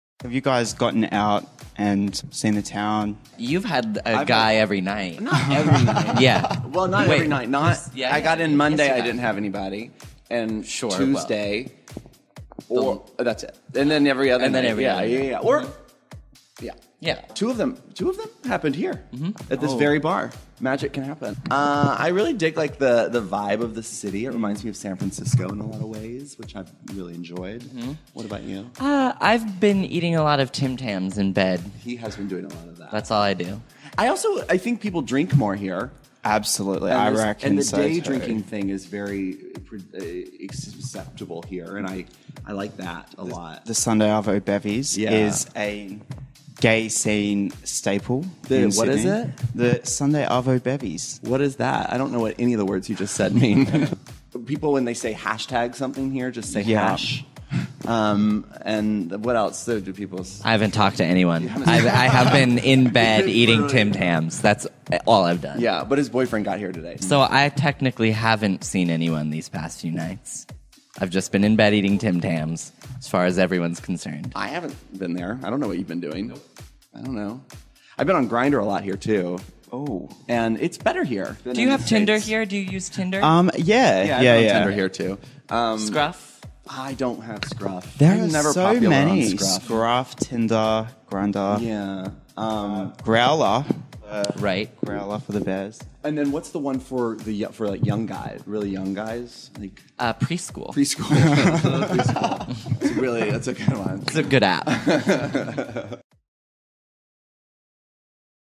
Jeffery & Cole in Sydney, Australia
YouTube stars Jeffery Self & Cole Escola talk to OX Live about their experiences with Aussie boys!